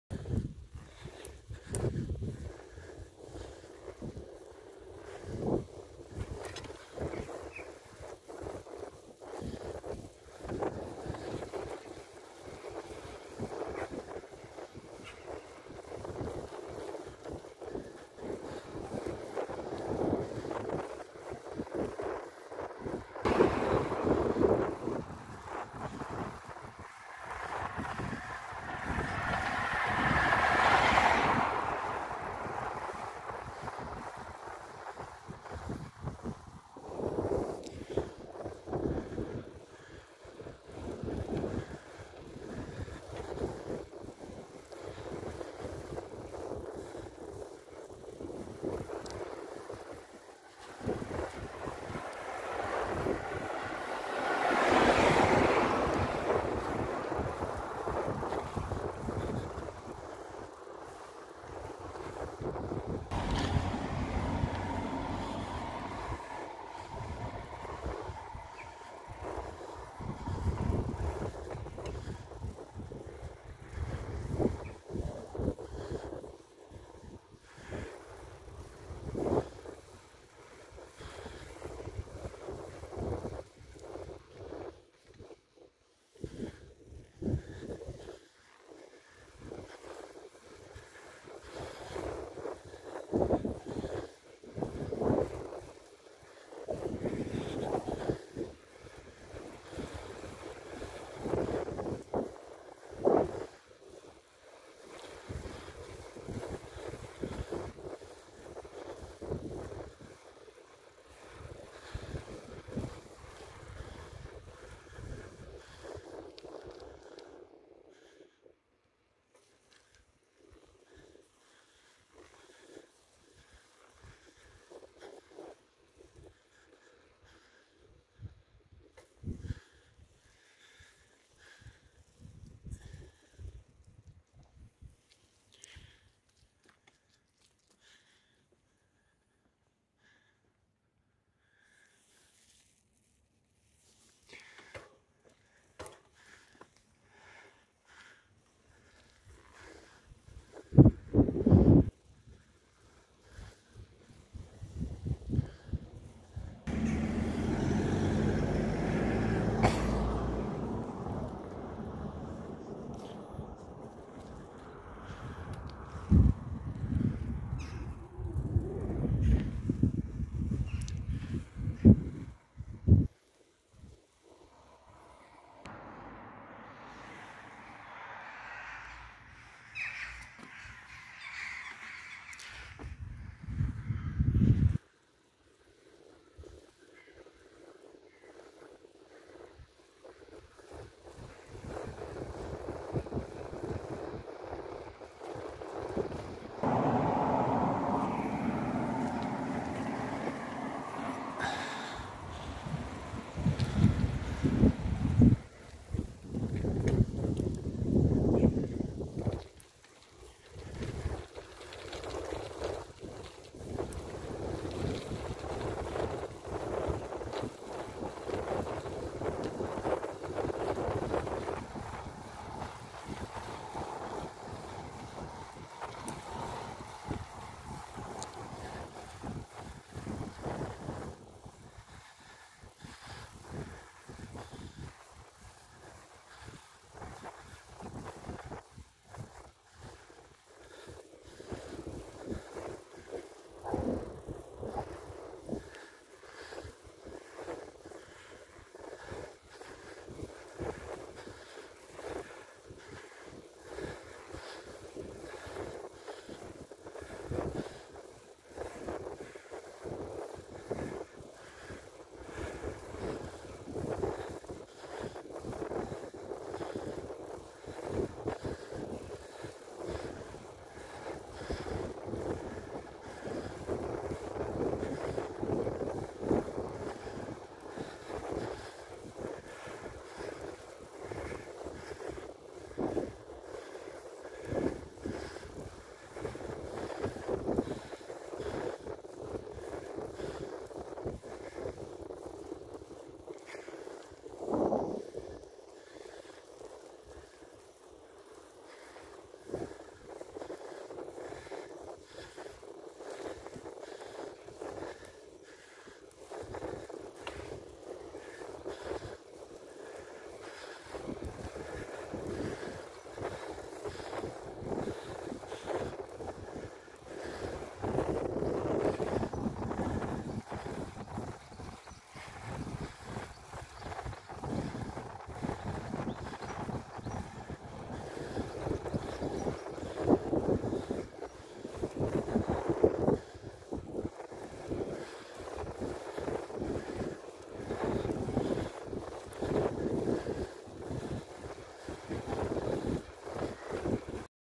A ride through Macroom on 2.8.2021
A ride through the centre of Macroom after 6am on the August Bank Holiday Monday morning 2.8.2021